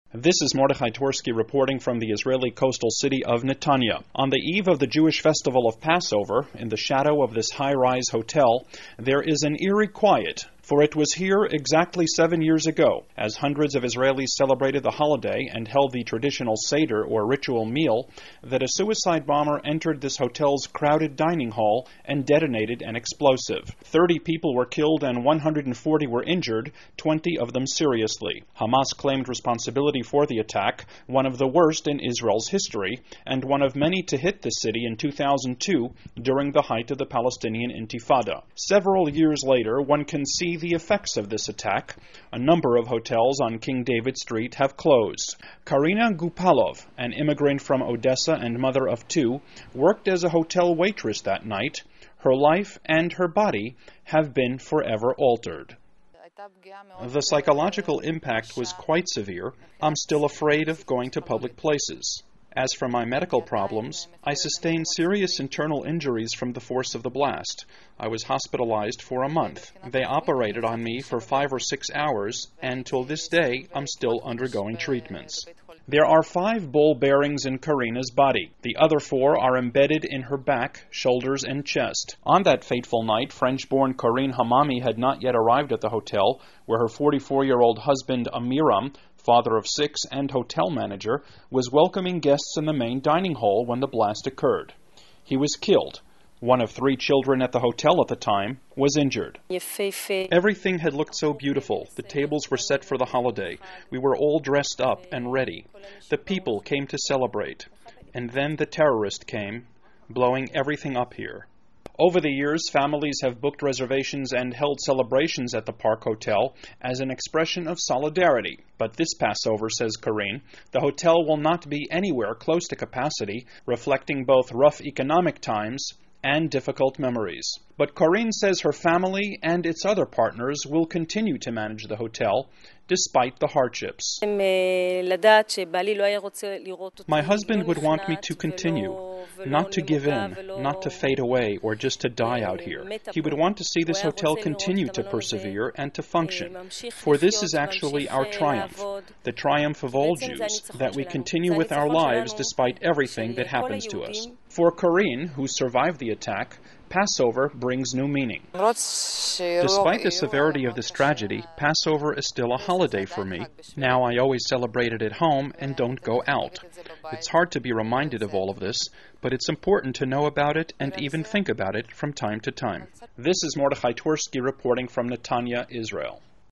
Seven Years After Passover Terror Attack, Survivors Remember, And Persevere
Passover eve marks the beginning of the Jewish festival of freedom.

But for some Israelis it is a time to remember a brazen terrorist attack against the Jewish state.

Seven years ago at the Park Hotel in Netanya, as hundreds of guests gathered in the dining hall for the traditional seder, or ritual meal, a suicide bomber entered and detonated an explosive.

Thirty hotel guests were killed, and 140 were injured.

Reporter Mordechai I. Twersky visited the Park Hotel a few days before Passover 2009, which begins tonight at sundown.

There he met a woman who survived the blast, and another, a hotel co-owner, whose husband was murdered that evening.

Soon after the blast the hotel was rebuilt. But attendance-wise, things just haven't been the same. It won't be filled to capacity this holiday, a victim of economic hard times and difficult memories.